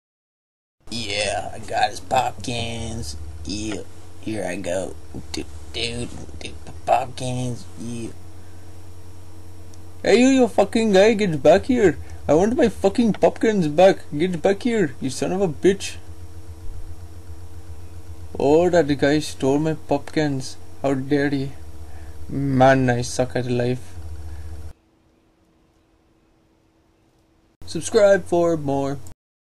Steal my POP CANS!!!
A bum stole some pop cans off a neighbor, so this east Indian man tried to chase him but failed!!